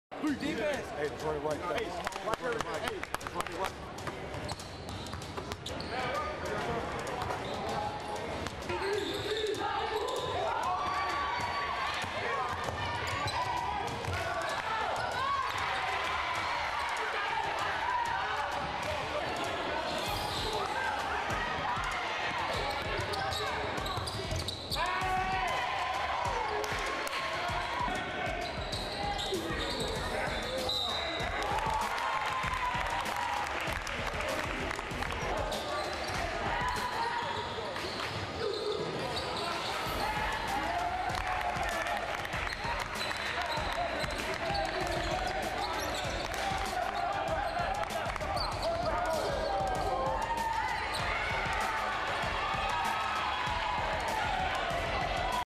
Southeast Bulloch vs. Savannah: 2019 Region 3-AAA Tournament
Southeast Bulloch 43, Savannah 40